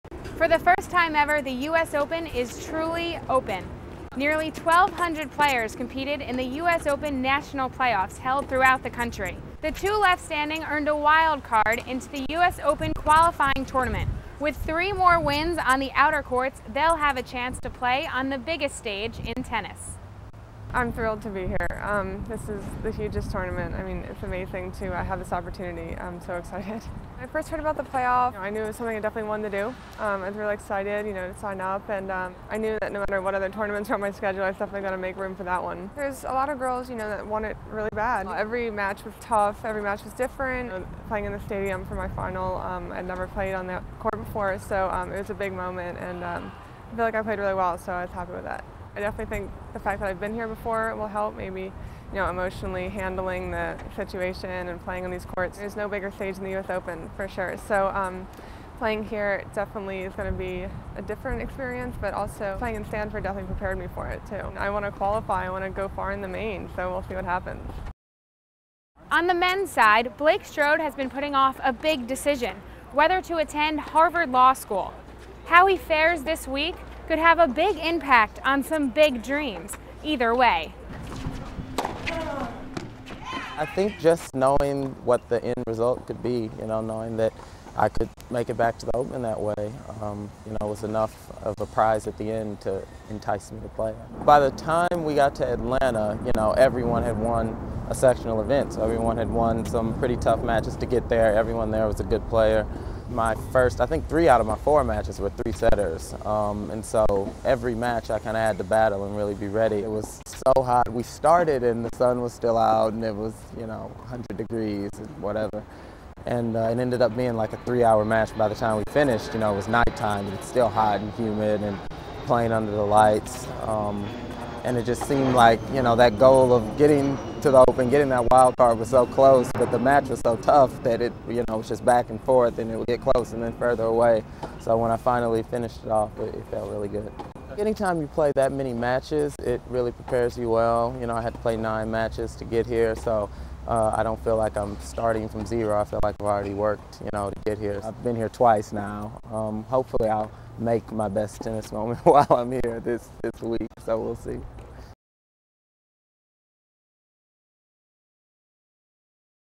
2010 US Open Qualifying Tournament: Meet Alexandra and Blake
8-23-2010

In anticipation of the 2010 US Open, the US Open Qualifying Tournament from August 24-27 showcases some of the finest young talent from around the globe all vying to make their way into the main men's and women's singles draws.

Two such hopefuls are 22-year-old Alexandra Mueller of Abington, PA and 23-year-old Blake Strode of St. Louis, MO - earlier in the summer, both players won the inaugural US Open National Playoffs in the women's and men's competition, respectively.

The US Open National Playoffs were held from April - June of 2010, in 16 different USTA sections across the nation. From teenagers to senior players, a field of over 1,200 participated for the right to make it to Queens... and now, two remain.

Will their long road lead to playing in their very first US Open?

WATCH THE 2010 US OPEN!

Broadcasted also on CBS, ESPN2 & The Tennis Channel